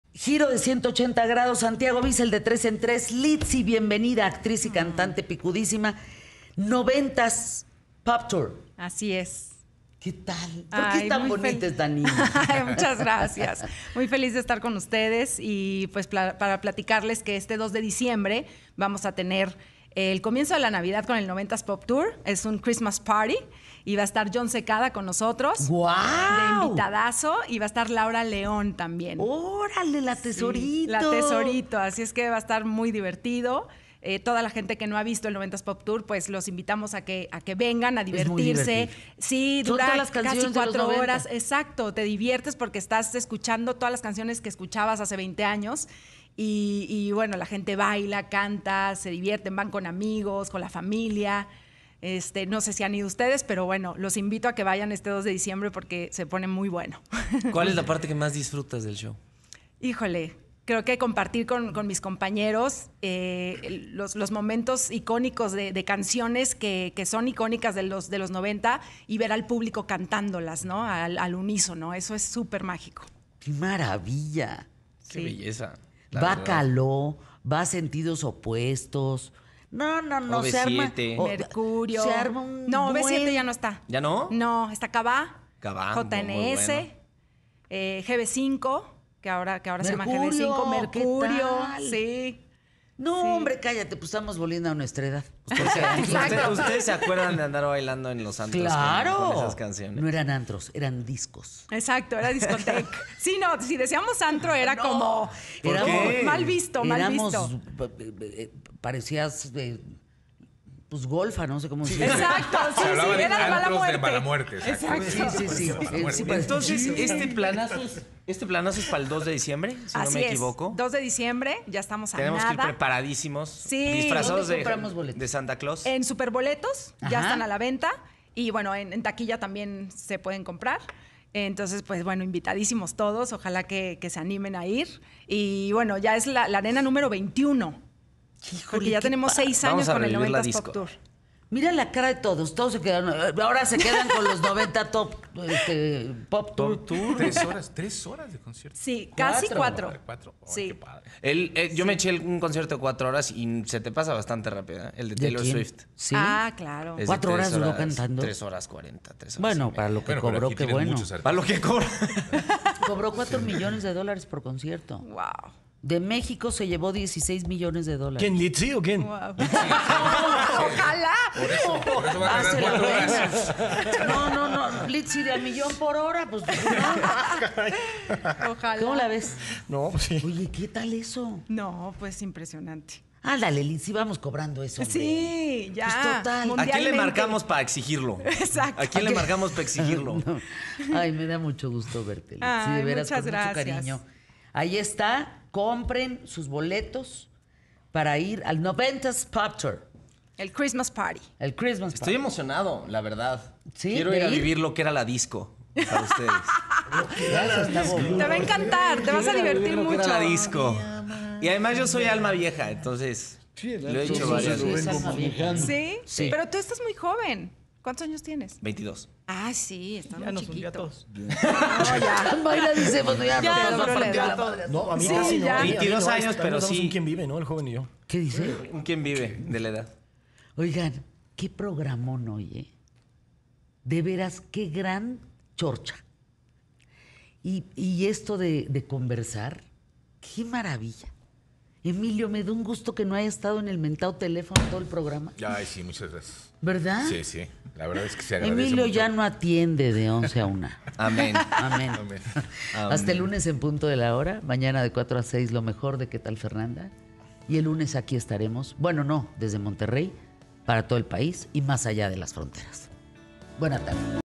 Litzy y el 90's Pop Tour en la Arena CDMX
La cantante Litzy y varias de las agrupaciones más populares del pop español en México se darán cita en la Arena Ciudad de México este 2 de diciembre.      24 noviembre de 2023